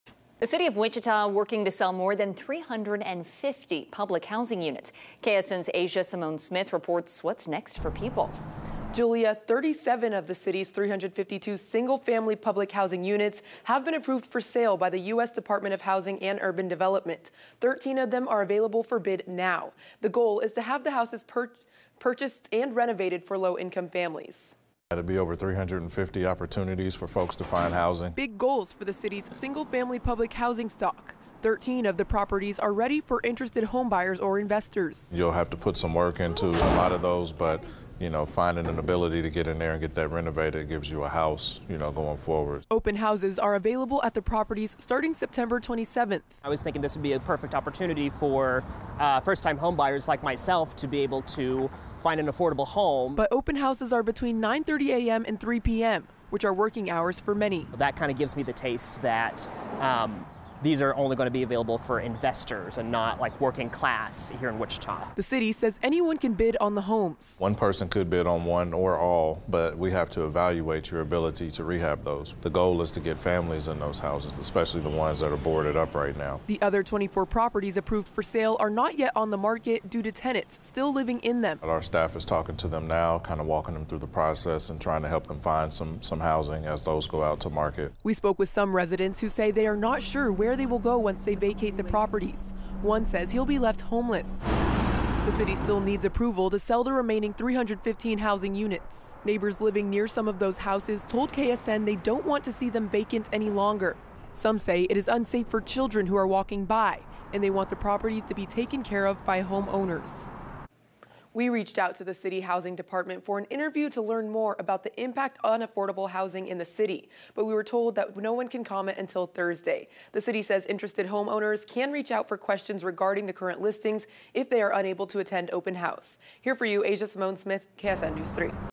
Wichita selling 13 public housing units
The City of Wichita is selling 13 public housing units and is giving potential buyers a chance to tour the homes this week